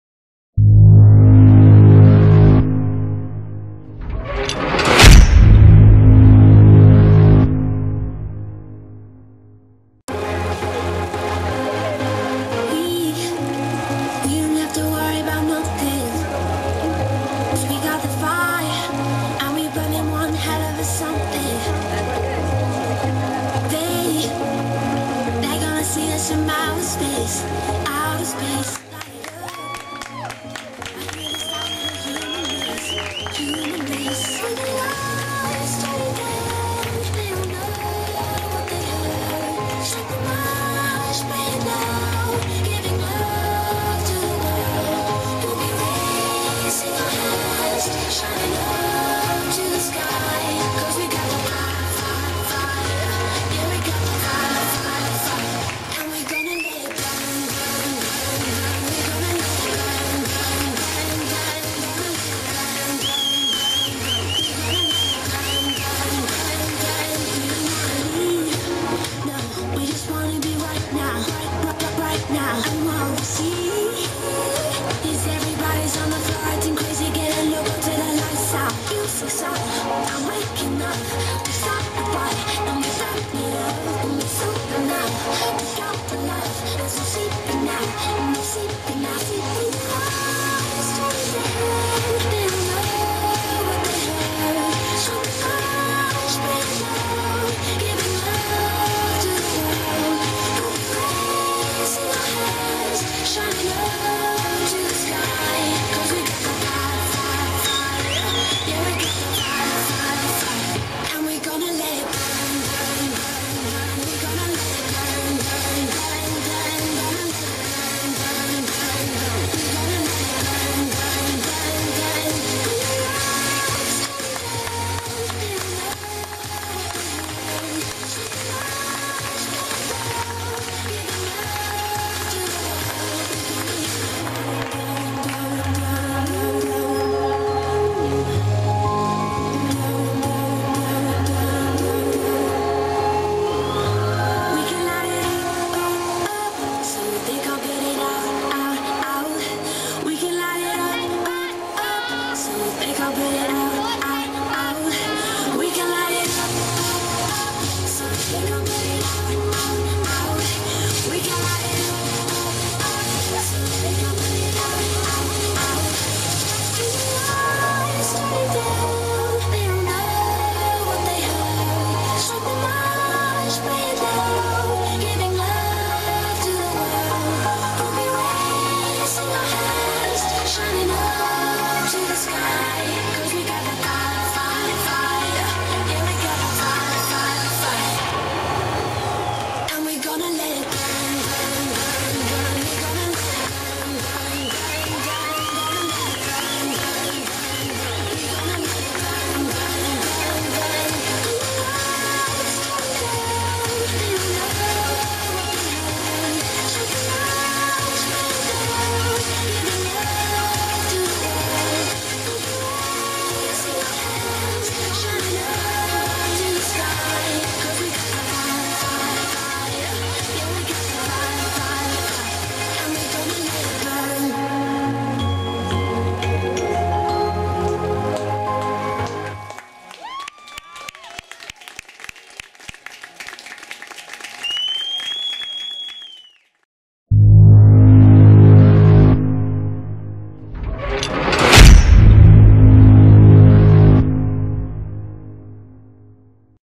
Piroland 90 - Sparkular taglio torta
Scenografia di fontane a tempo di musica per i nostri Sposi

Richiedi il Tuo Show per far rimanere tutti a bocca aperta!